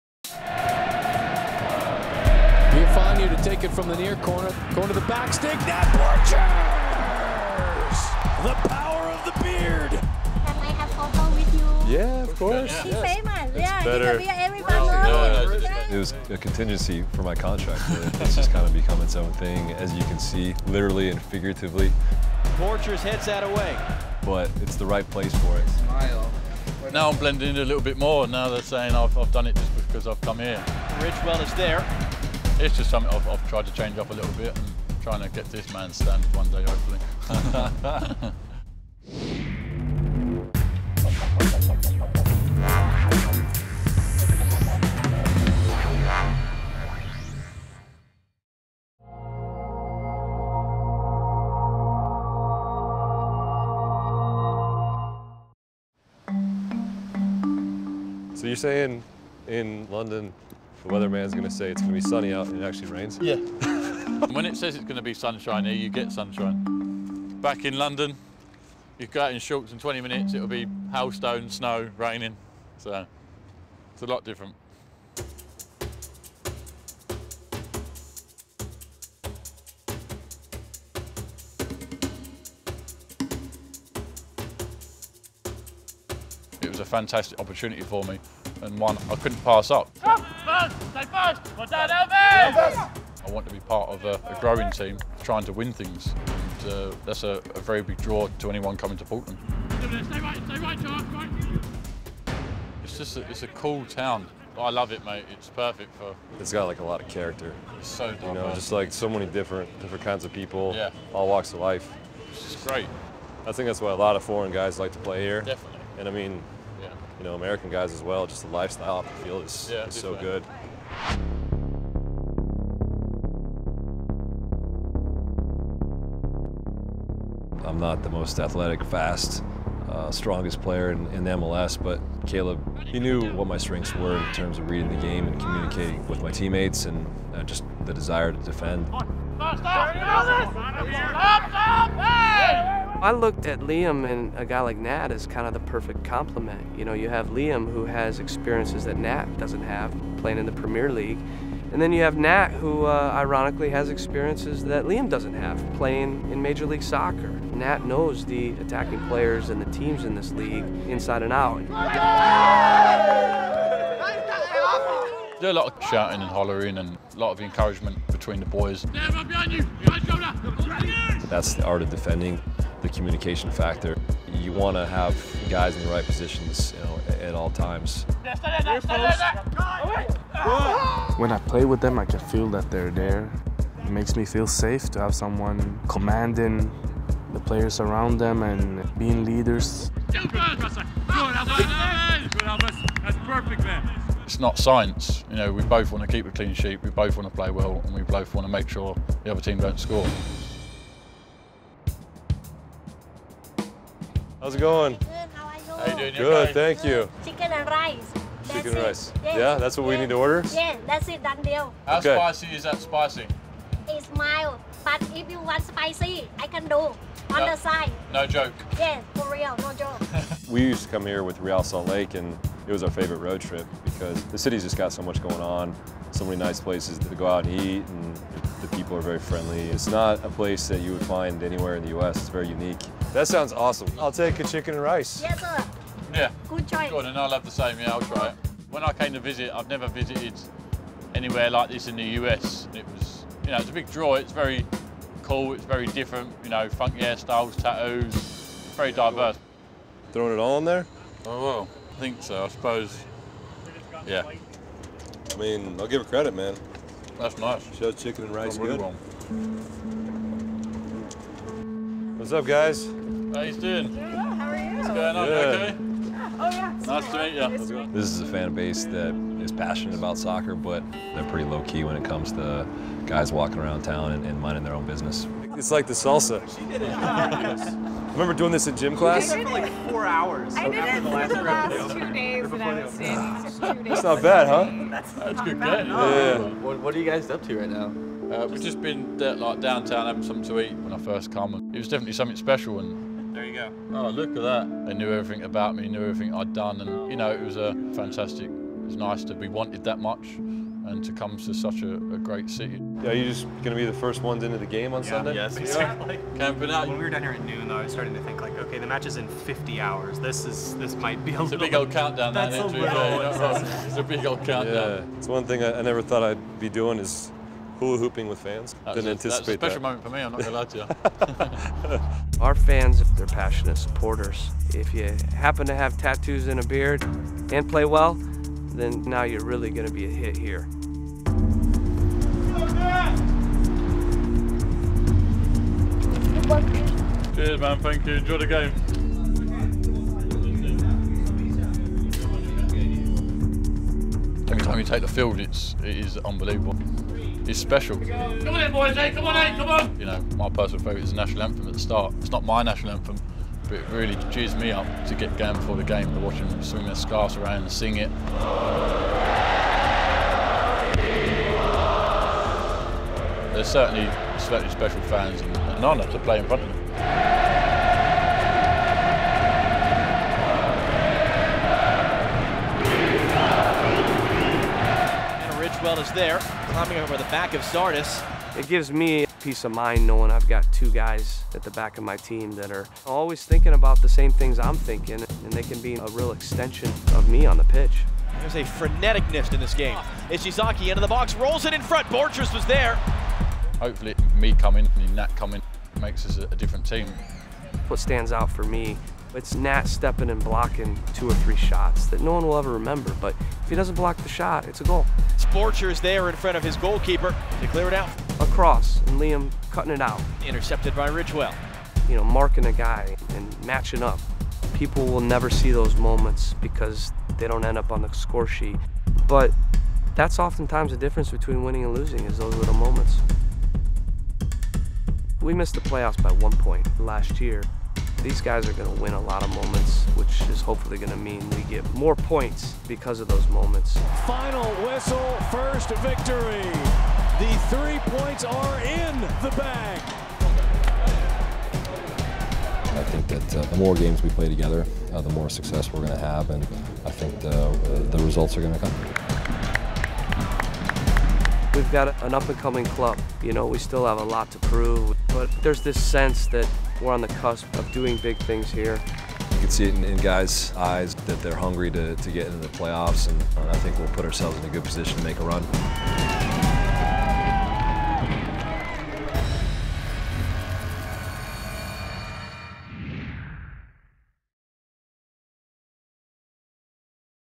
Liam Ridgewell & Nat Borchers Are Defending Portland | MLS Insider
The Portland Timbers brought in two experienced center backs to anchor their defense: Liam Ridgewell and Nat Borchers. The bearded duo have embraced their new city's unique culture and endeared themselves to fans.

MLS Insider Presented by adidas dives into the story lines behind Major League Soccer. Your favorite players. Your favorite teams. Unprecedented access.